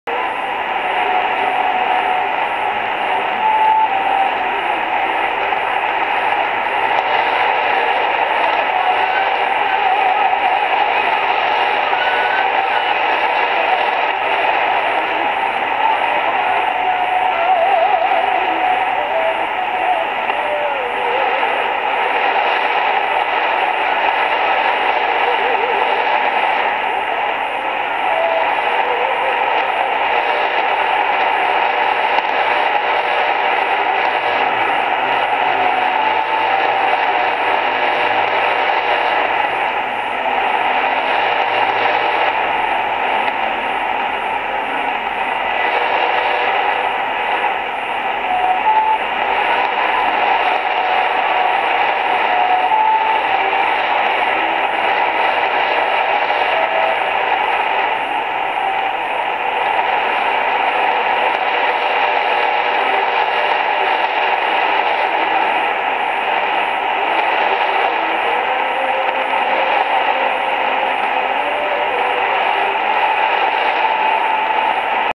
4850 khz PBS Xinjiang (Urumqi Changji)
4850  PBS Xinjiang
February 23 2019 / 01:23 UTC
Kazakh
PWR 100 AZ ND
TX Site Urumqi Changji
RX Site Belo Horizonte
Receiver Sony 7600gr
Antenna: EF-SWL (End-fed)